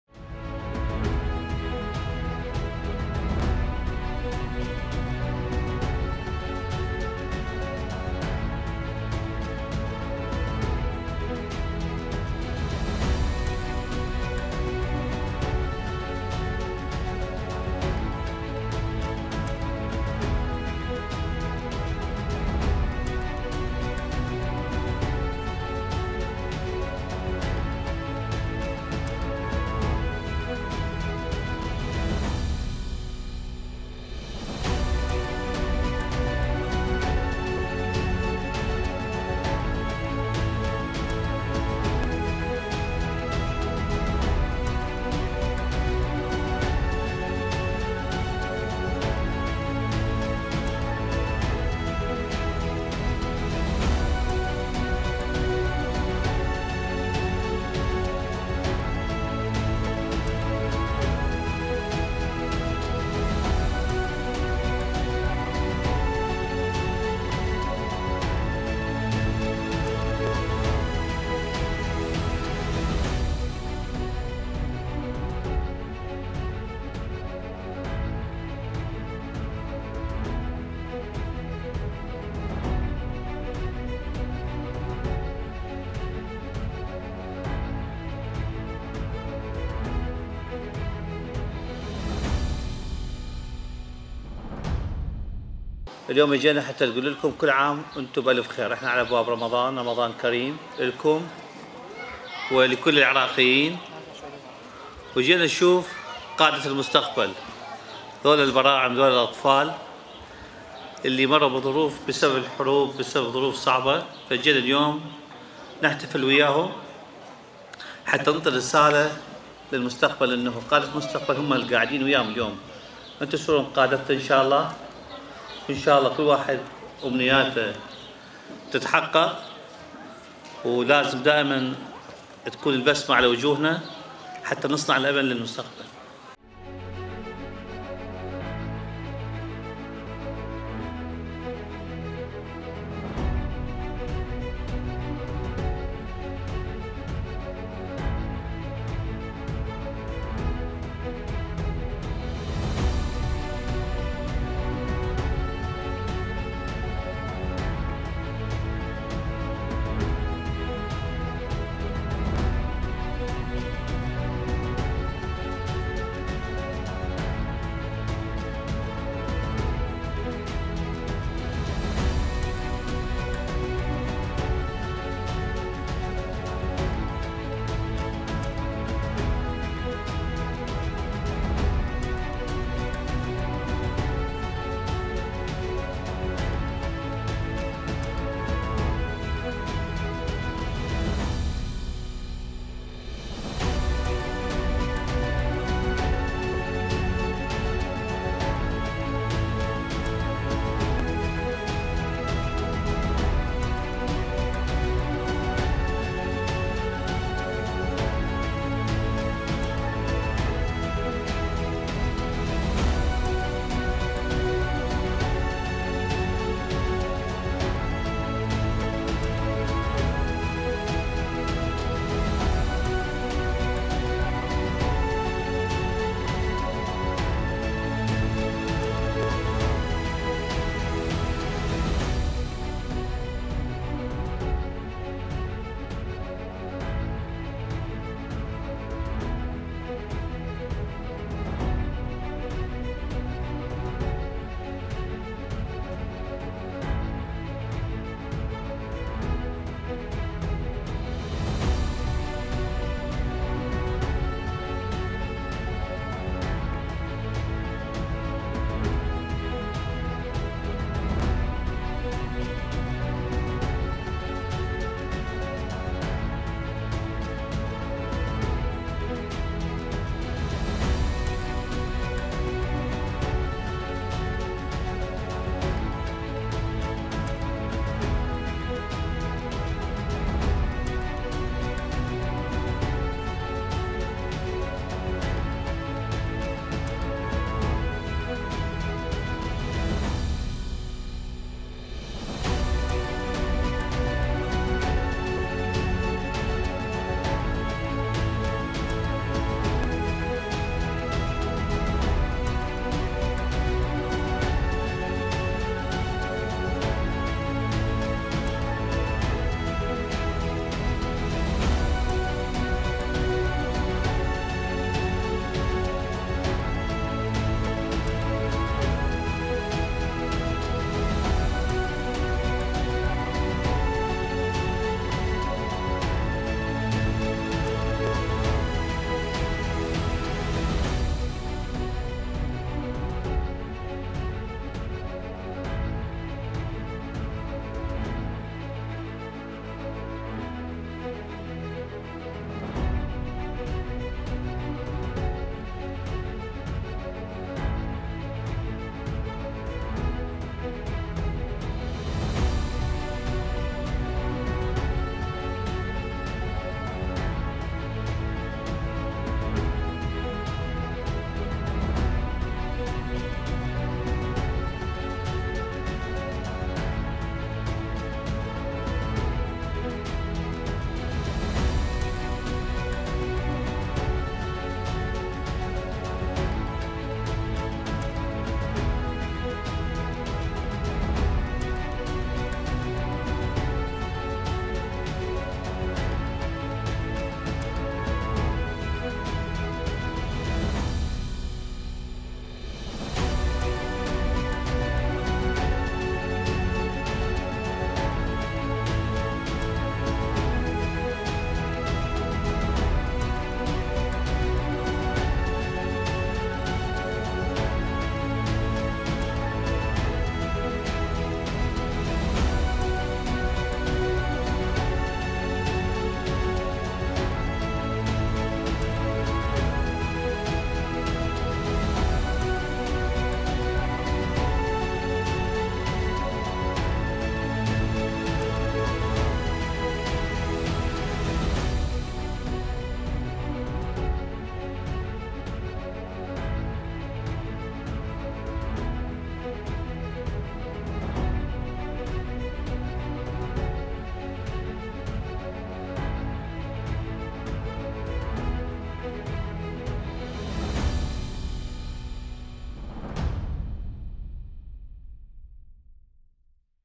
رئيس مجلس الوزراء يحتفي بمجموعة من الأطفال الأيتام بمناسبة اسبوع اليتيم العراقي ويوم اليتيم العربي
رئيس مجلس الوزراء السيد مصطفى الكاظمي يحتفي بمجموعة من الأطفال الأيتام بمناسبة اسبوع اليتيم العراقي ويوم اليتيم العربي